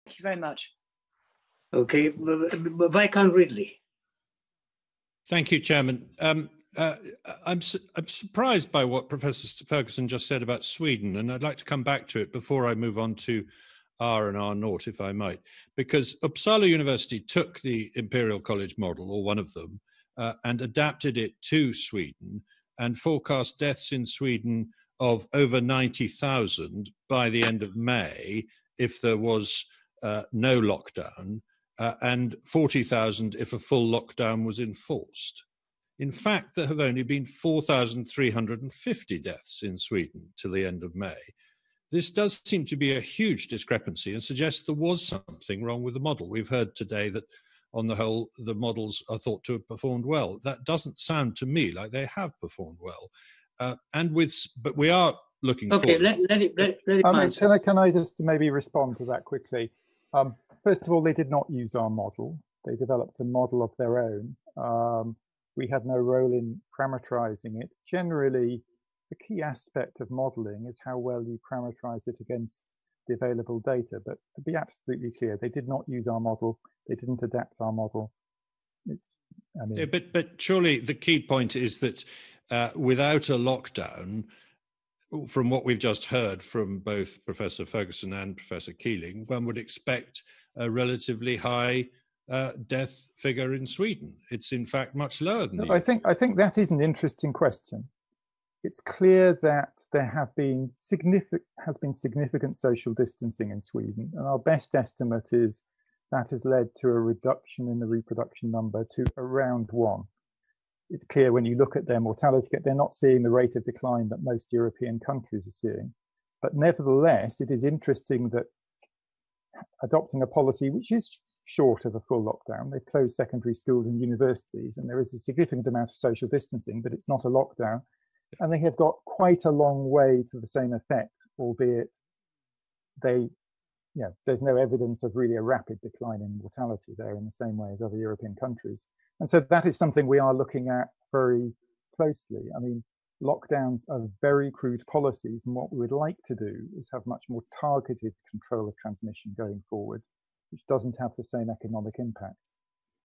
Matt Rildey questions Neil Ferguson over the Imperial Model
Ferguson appeared before the House of Lords Science and technology select committee on 2 June 2020.